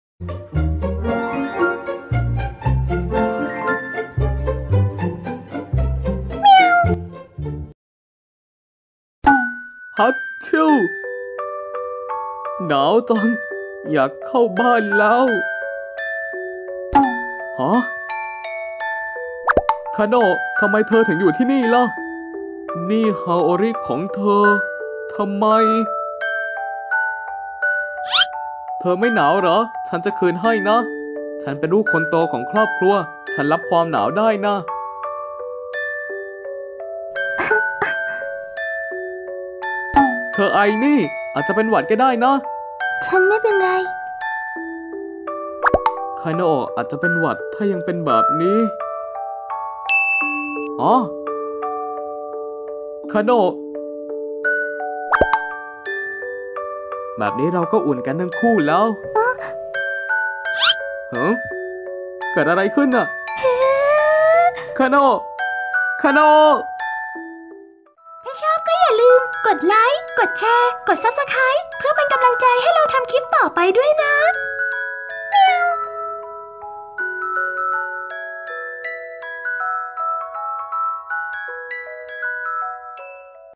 [ฝึกพากย์] ความหนาวเป็นเหตุ // คานาโอะ x ทันจิโร่ Kimetsu no yaiba - Kanao x Tanjiro
ฝึกพากย์โดย
Hanyuu - คานาโอะ
Can2772 - ทันจิโร่

เครดิตรูปภาพฝึกพากย์
Original Artist : ak_qjj (Twitter)

แปลโดย : Play it cool